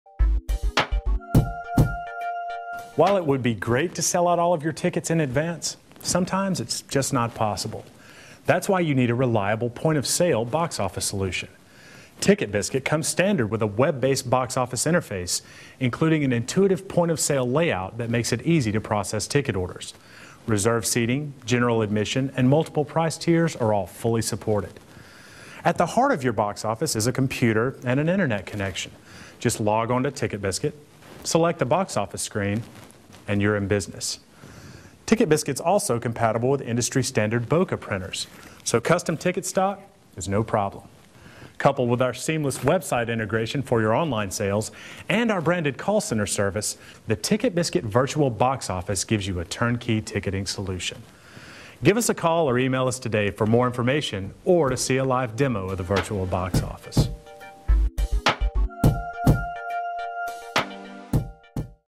Box Office Software from TicketBiscuit
In addition to our leading online ticketing service, TicketBiscuit makes a full-featured box office interface available to all of our clients. The box office is web-based, meaning that any computer with an internet connection can function as a box office terminal.

Coupled with our online interface and our branded call center service, the TicketBiscuit web-based Box Office can meet all of your ticketing needs. Give us a call or email to learn more,